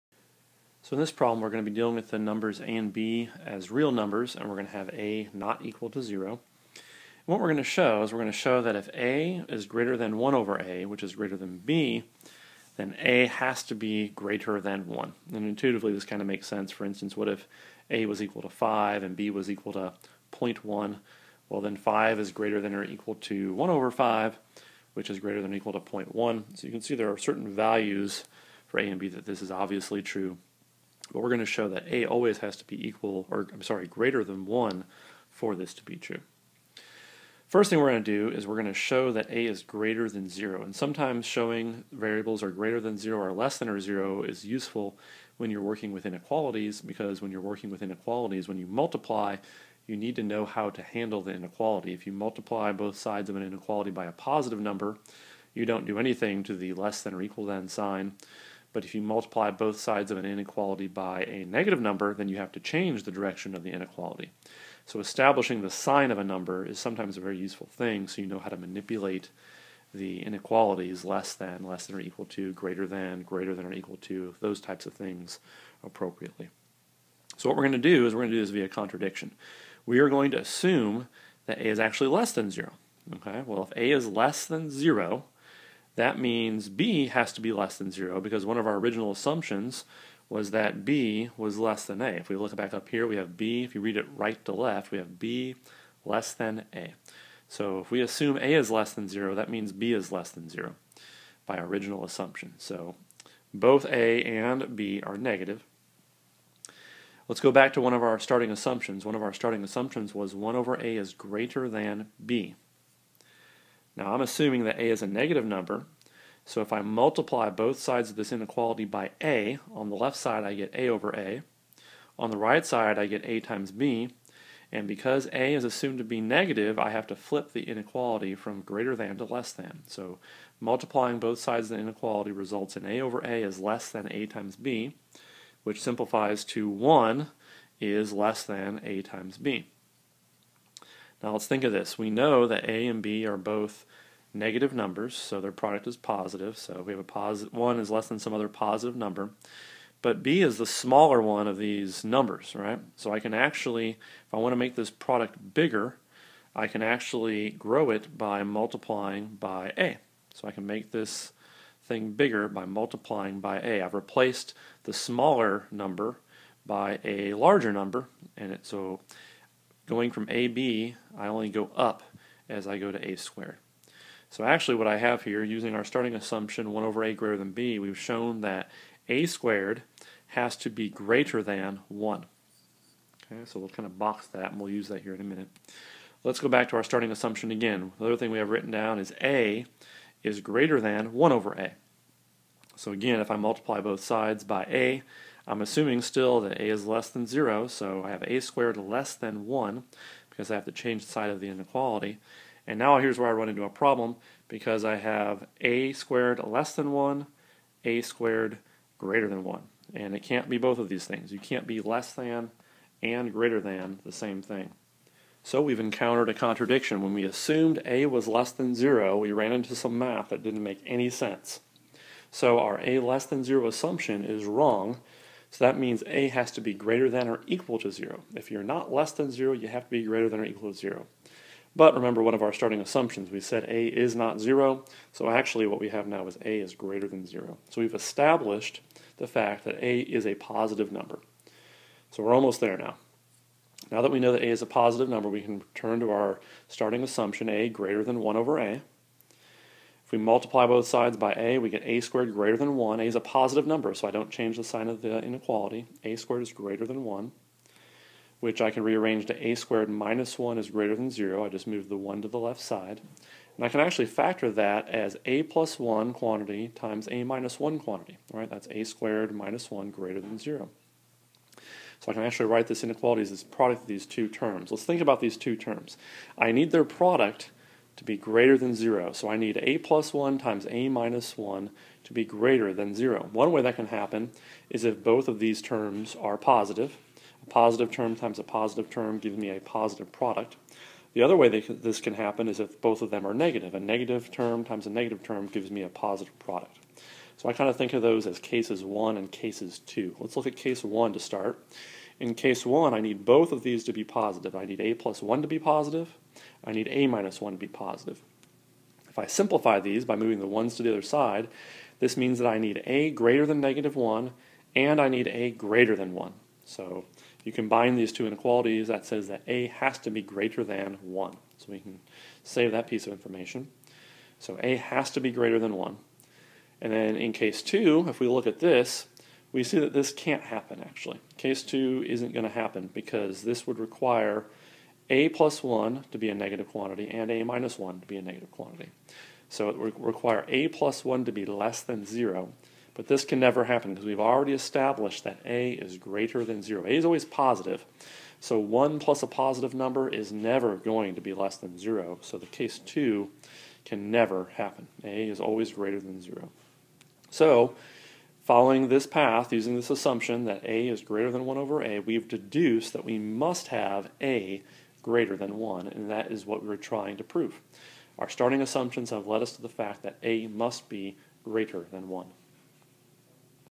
Proof and Problem Solving - Contradiction Proof Example 02
This problem works a simple proof using the method of contradiction.  Assuming "a" and "b" are real-valued and not equal to zero, we show that if "a" greater than 1/a greater than b, then "a" greater than 1.